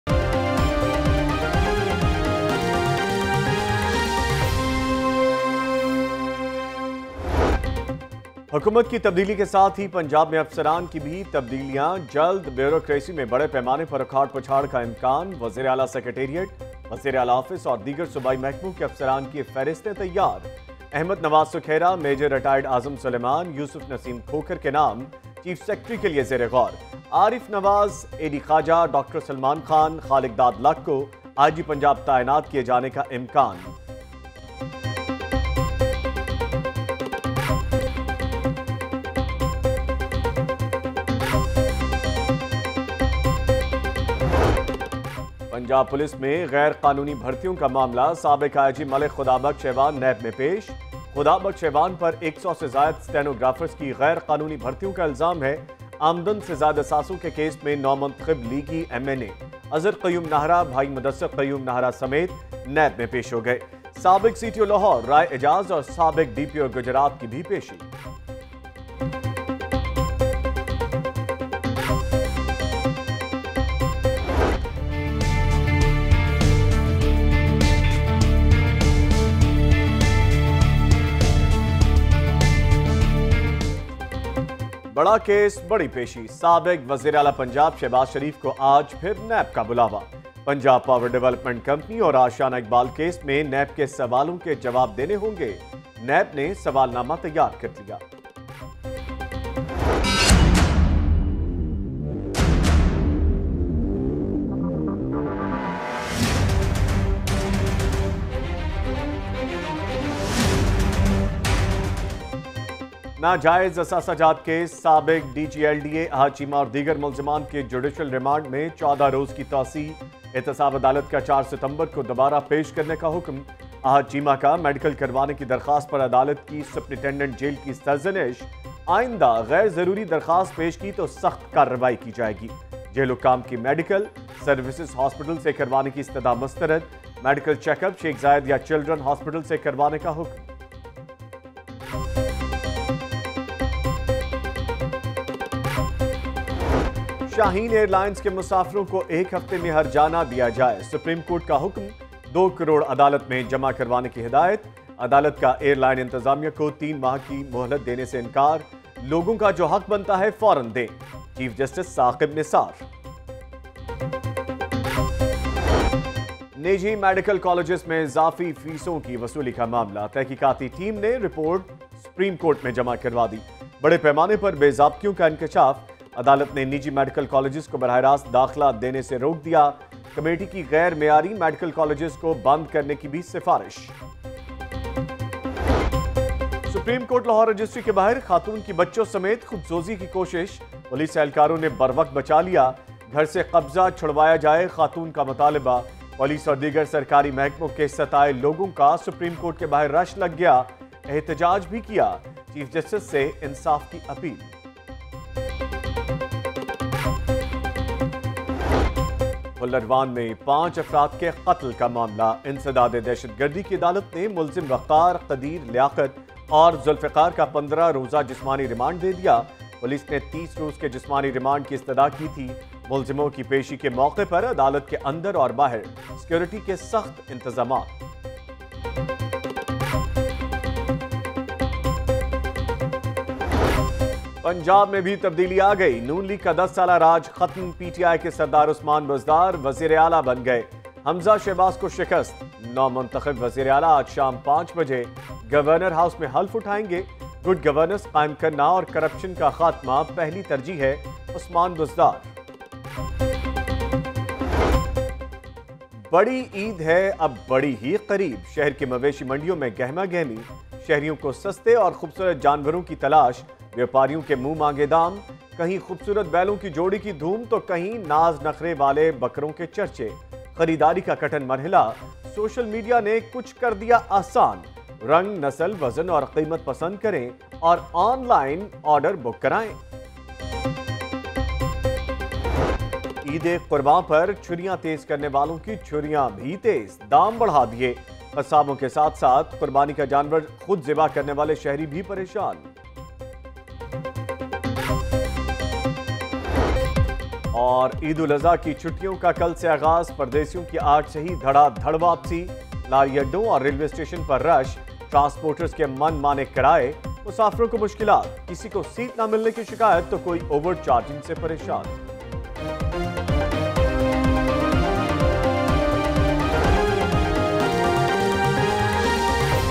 01 PM Headlines Lahore News HD - 20 August 2018
Watch 01 pm headlines: Lacs of Muslims to perform Hajj - Chief Justice to hear important cases in Supreme Court Lahore Registry today - NAB summons Shehbaz Sharif today - Imran Khan's first speech - Imran Khan's agenda all set - Will end corruption and bring money back to Pakistan, says Imran Khan - PMLN Raj in Punjab ends after 10 years - Usman Buzdar to take oath in Governor house today - Punjab cabinet's names released - Imran Khan to form Parliamentary commission to sort rigging accusations - Nawaz Sharif and Maryam Nawaz expected to get bail before Eid - Navjot Singh Sidhu roams around Lahore - Eid ul Azha draws near, citizens out in search of animals.